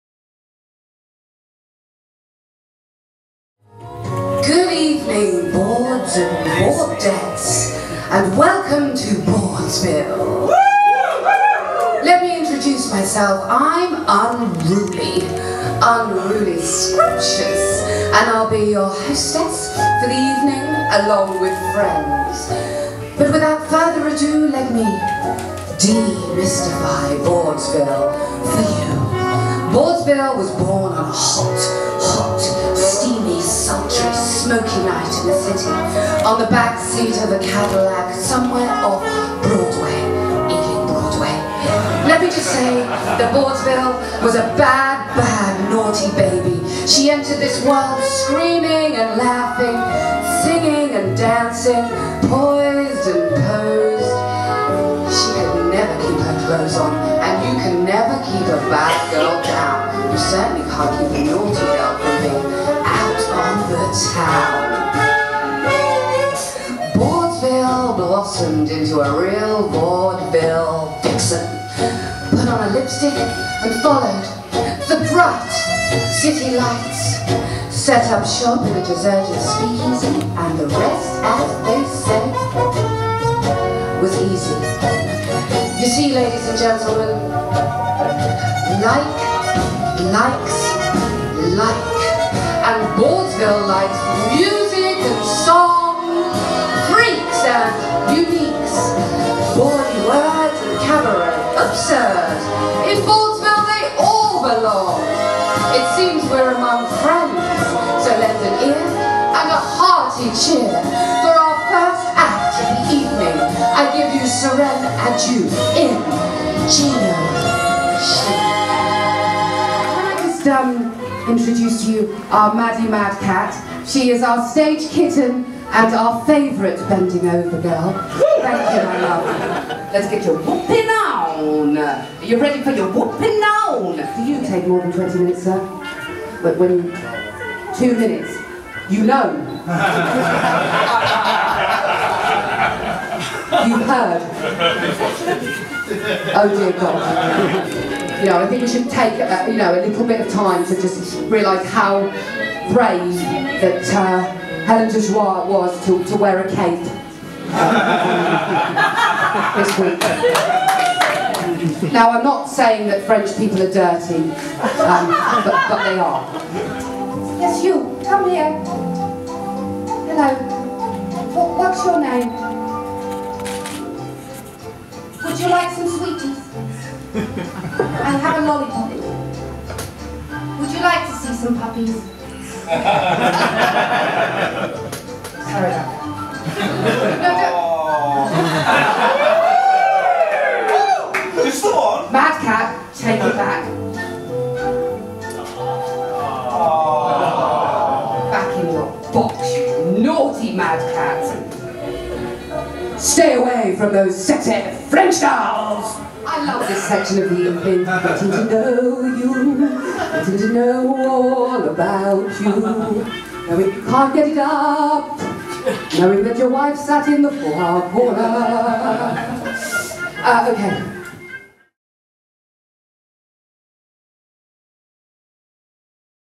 UNRULY Scrumptious - BAWDSVILLE Cabaret
Unruly Scrumptious Best Bit of Bawdsville in the Bush February 2015.

Unruly Scrumptious for Bawdsville Cabaret at the Bush Theatre Library - February 2015.